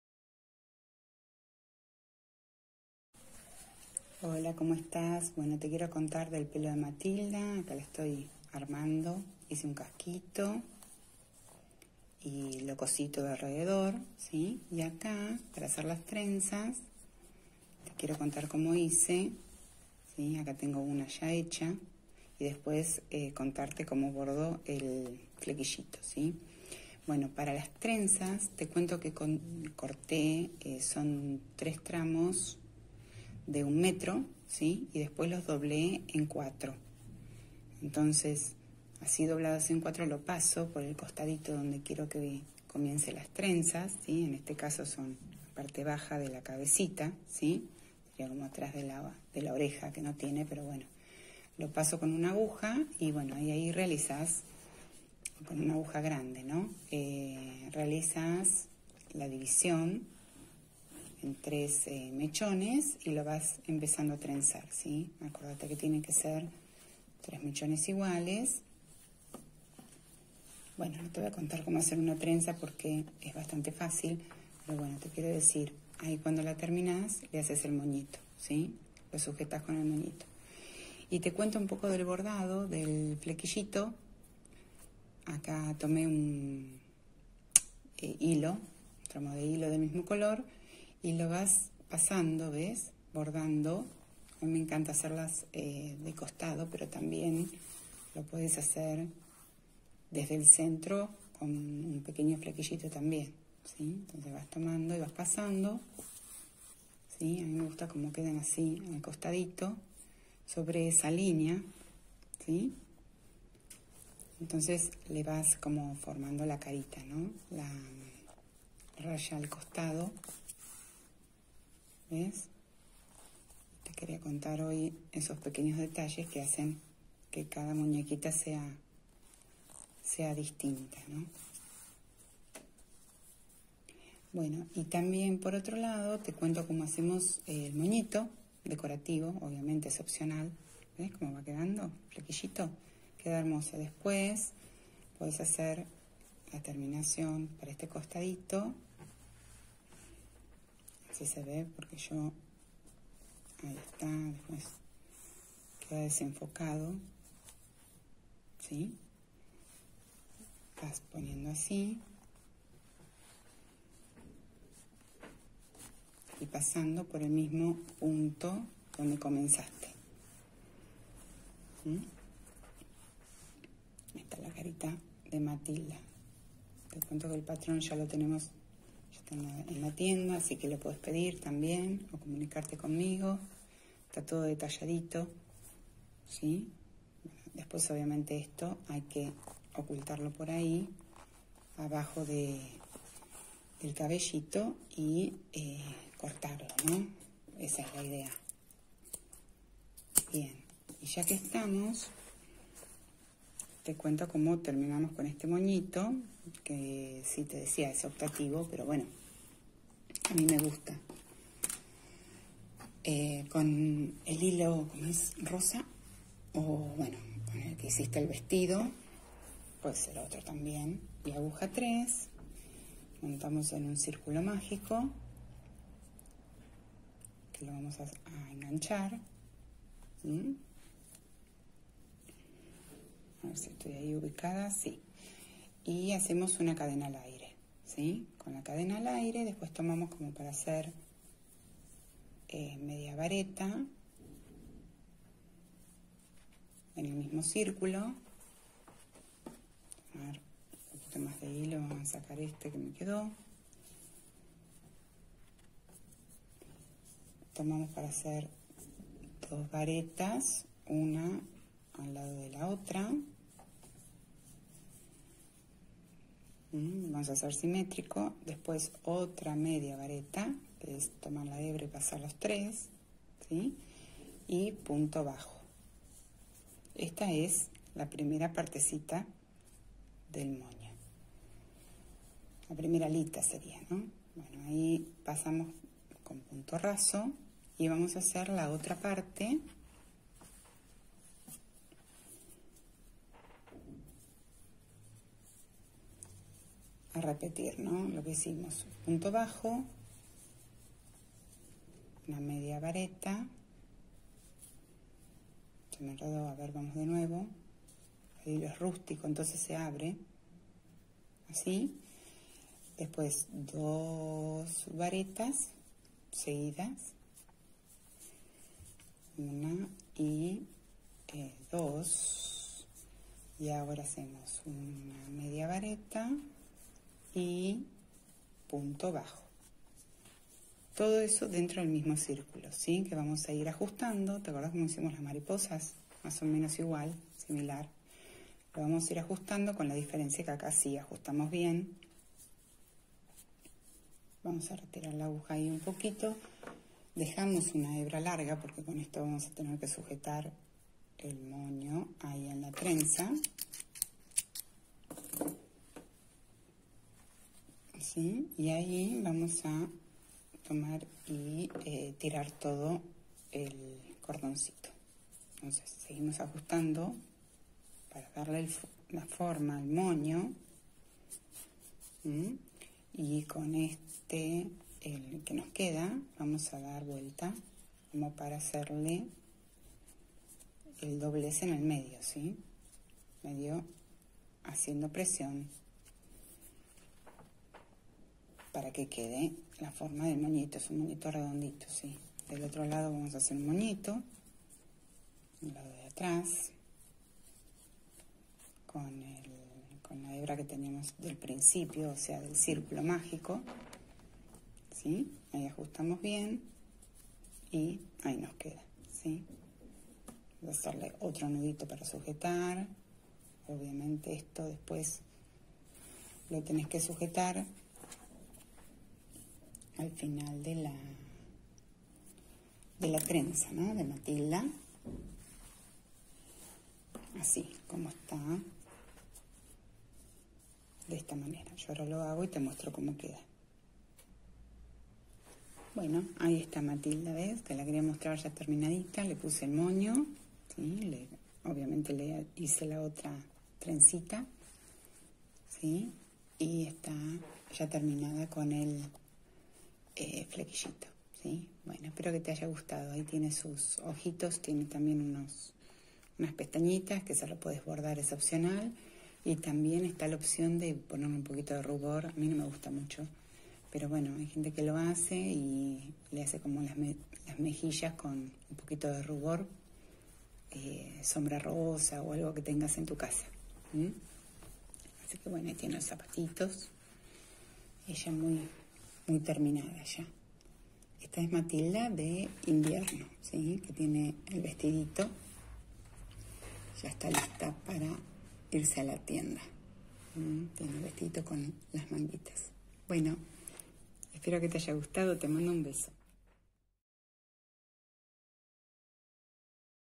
Tutorial : terminaciones de cabello y moños de MATILDA - AMIGURUMIS BY INES -
Hola! Te cuento cómo creo y hago los detalles en Muñecas
En este caso a Matilda la tejí en hilo de algodón rústico color rosa y te cuento cómo realicé el cabello y las trenzas,  el bordado del flequillo, los moñitos del mismo color que el vestido y todos los secretitos de ese proceso.
Las que hacemos amigurumis sabemos que son un trabajo de paciencia y dedicación y tienen un montón de detalles chiquitos para tener en cuenta y que queden bonitos...
Amo estas muñecas.. son de las primeros amigurumis que realicé y lo mejor que tienen para mí es que se pueden versionar de mil modos creando algo único cada vez que tejés una...
Encontrás en mi catálogo el patrón con todas las explicaciones para poder hacerla vos misma, tanto en version verano como en versión invierno, porque la versioné obviamente... no puedo con mi genio! 😅
Hay otros patrones y guías de tejido disponibles: Simona la Coneja, Nina la Sirena, Alberto Amigurumi, y otras.